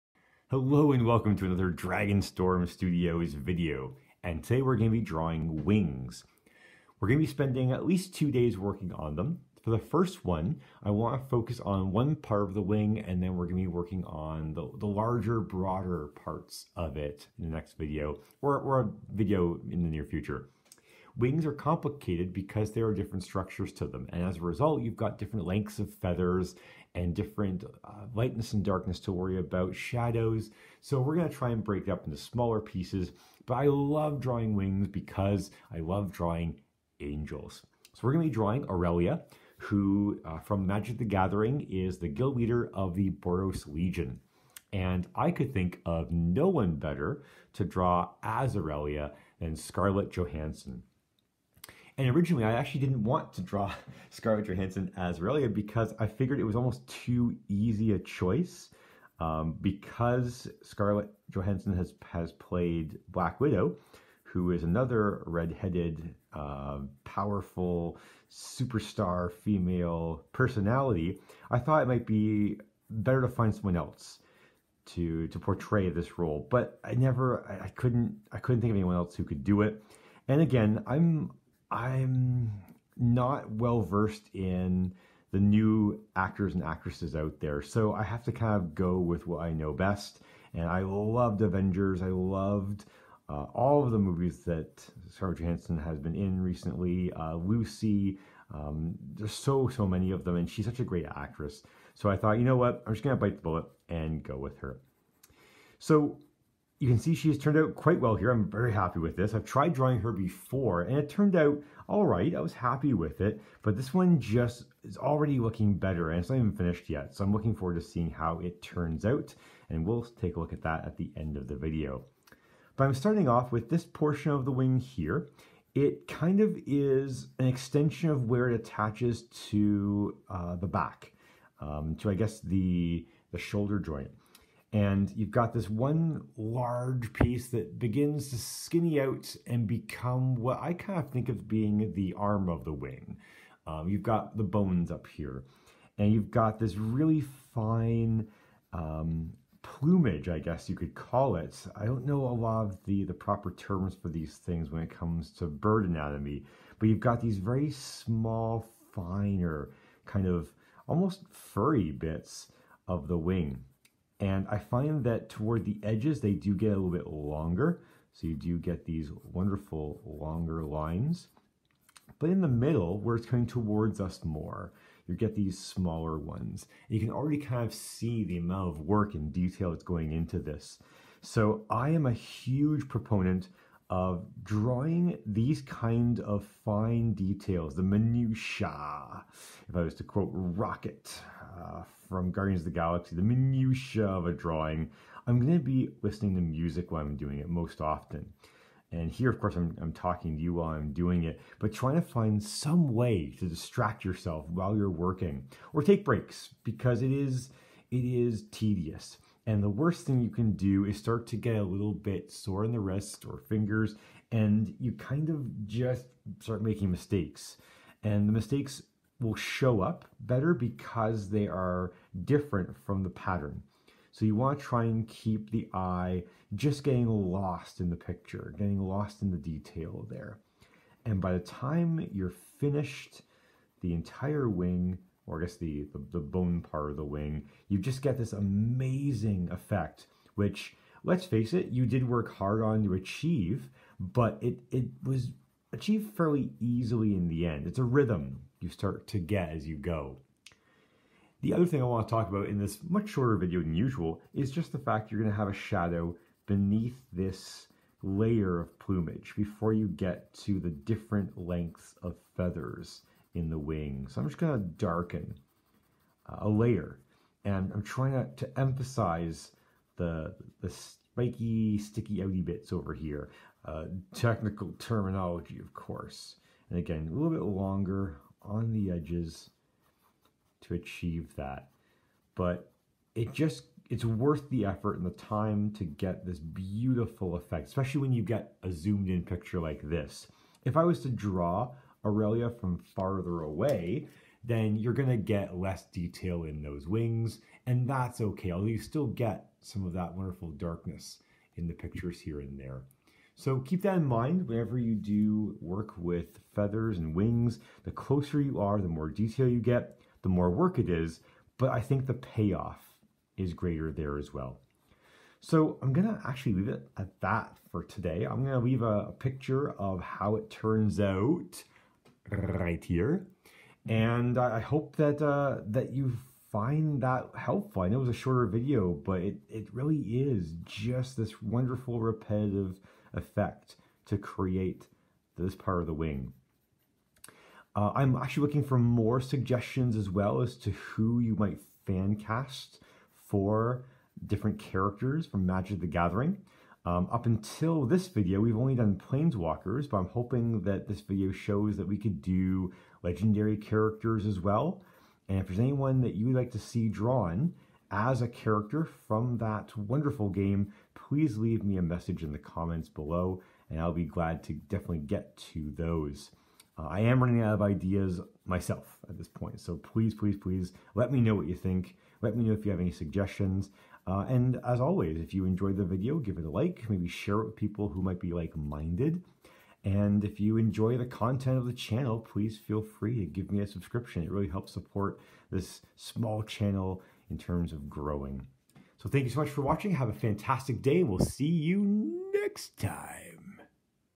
Drawing Scarlett Johansson as Aurelia
Reveal video of Scarlett Johansson as the Boros angel Aurelia. We also discuss drawing wings (part 1 of 2)

It turned out pretty well and I'm feeling more comfortable drawing the female face.